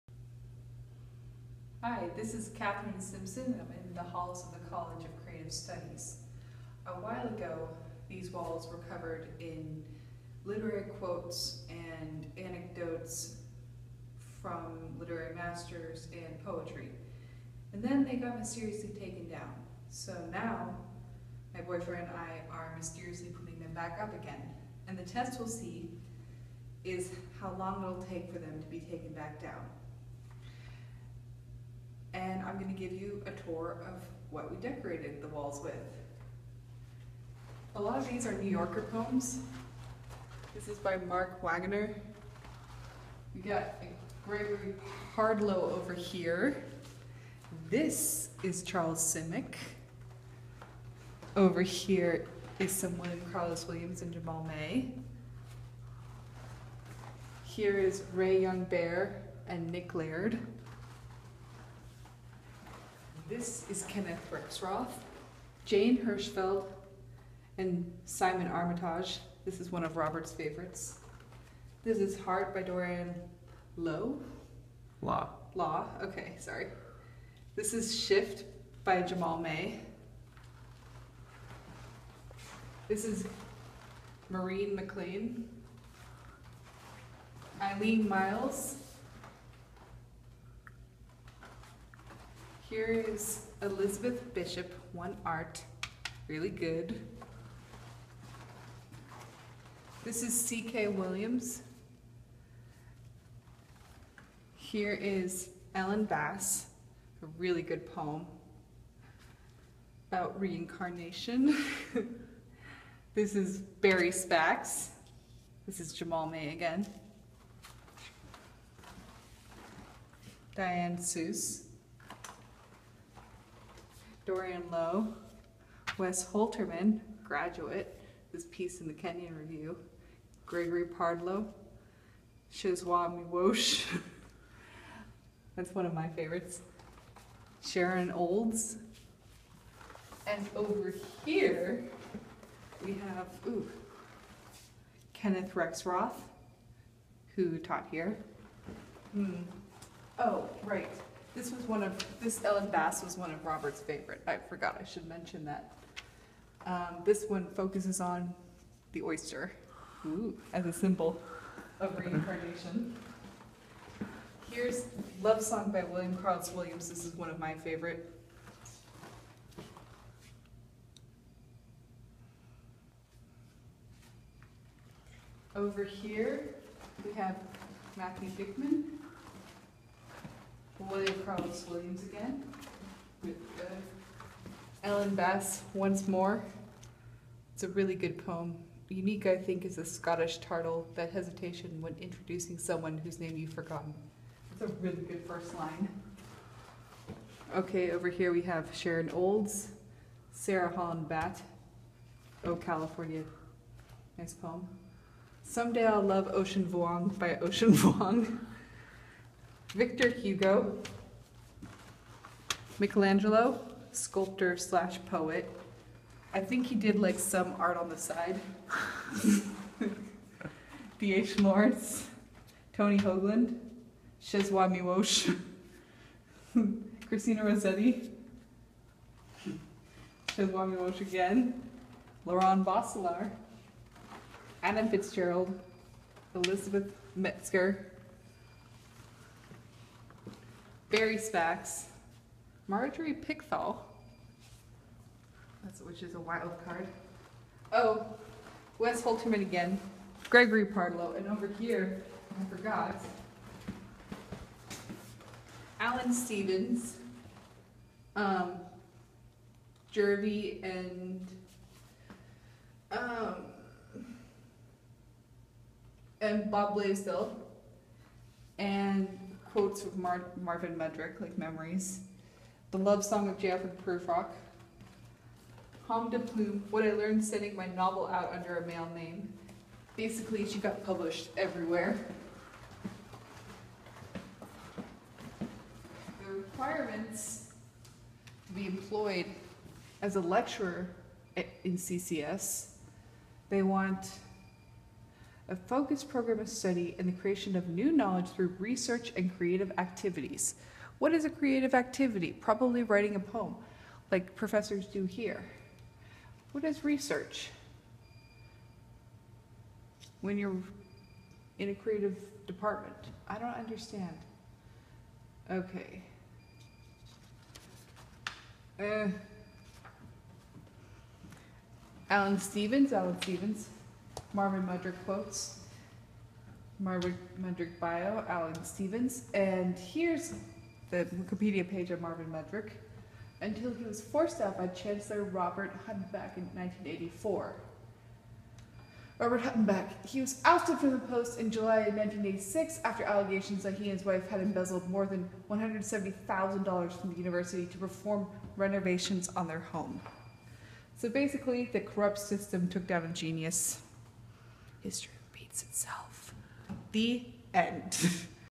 Reclaiming the Walls of CCS
Administrators took down the art and poetry from the walls of the College of Creative Studies. We put it back.